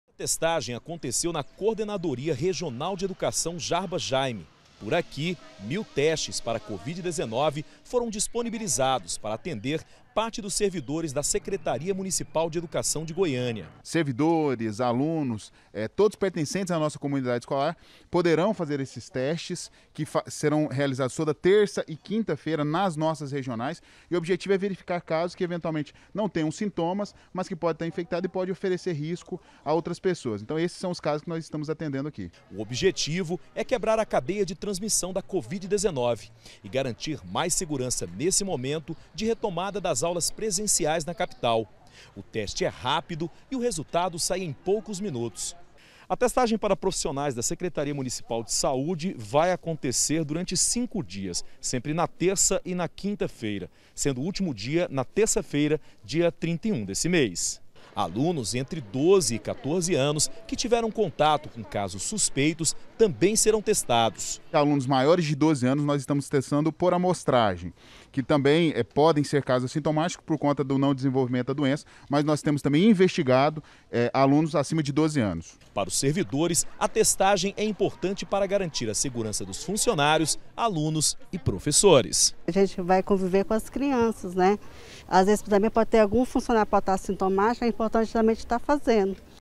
SEGURANÇA NAS ESCOLAS: PROFISSIONAIS DA EDUCAÇÃO SÃO TESTADOS EM GOIÂNIA.
COMEÇOU HOJE A TESTAGEM DOS PROFISSIONAIS DA REDE MUNICIPAL DE EDUCAÇÃO DE GOIÂNIA. FORAM EXAMINADAS 340 PESSOAS. DOIS TINHAM A COVID-19.